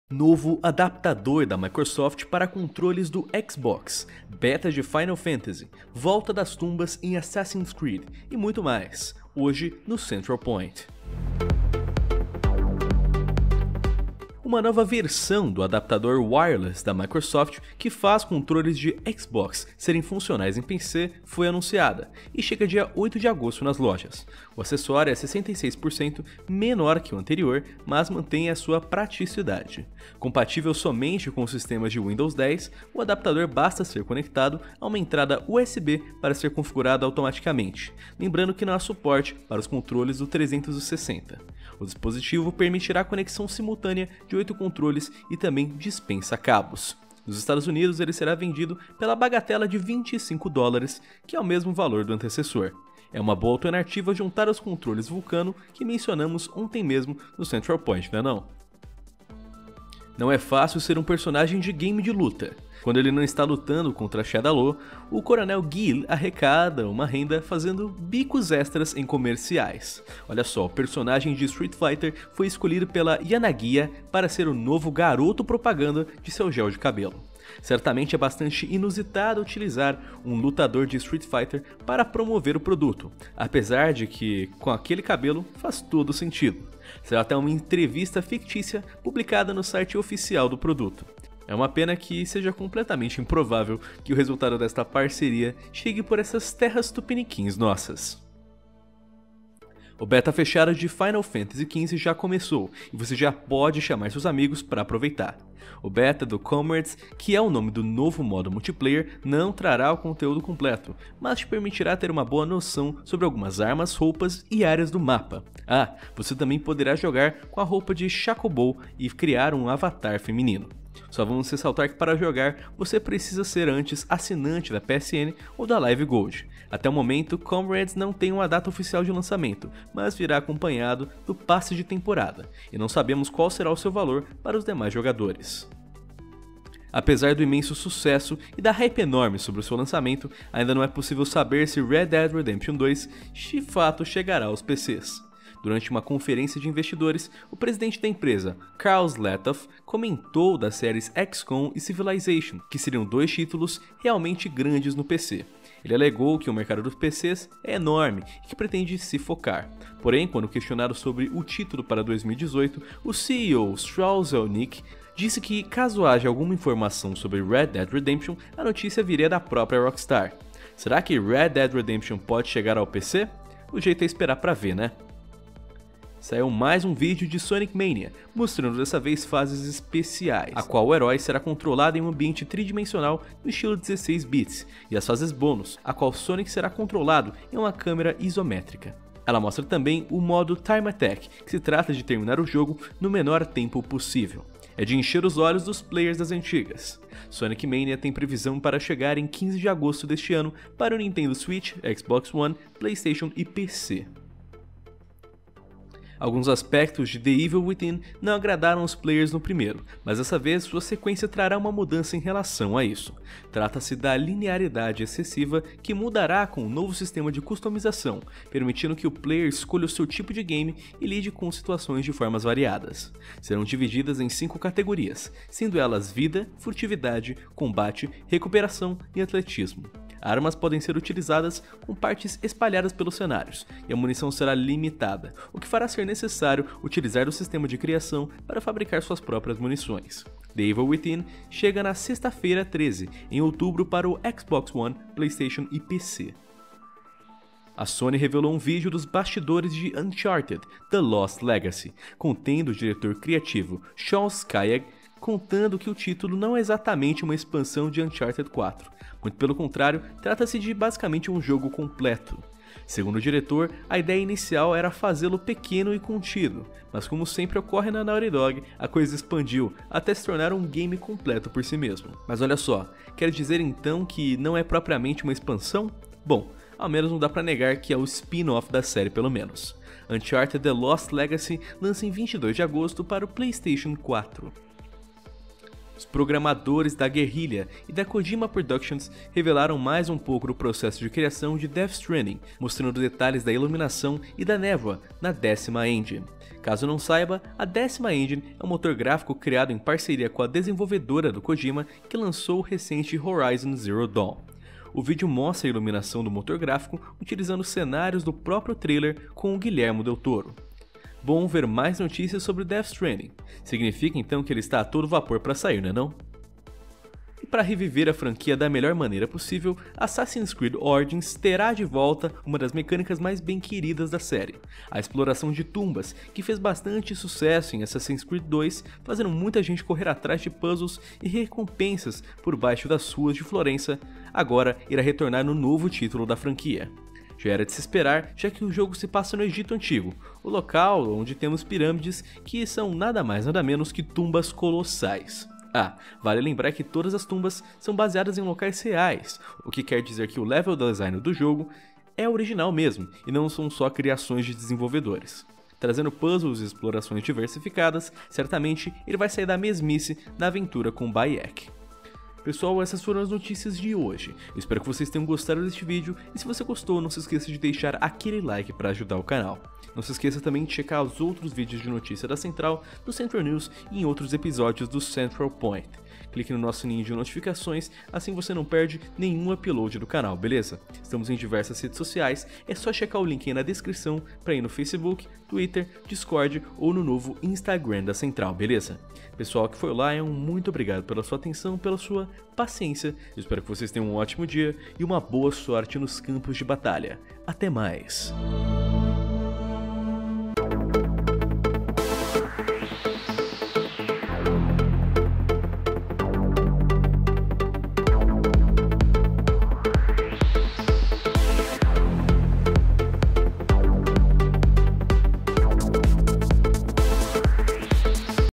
RED DEAD REDEMPTION NO PC? BETA DE FINAL FANTASY ONLINE E MUITO MAIS | CENTRAL POINT
Todo dia tem alguma notícia bizarra sobre o mundo dos games, e agora é hora de você conferir todas estas novidades, aqui no CENTRAL POINT.

(OVERWATCH)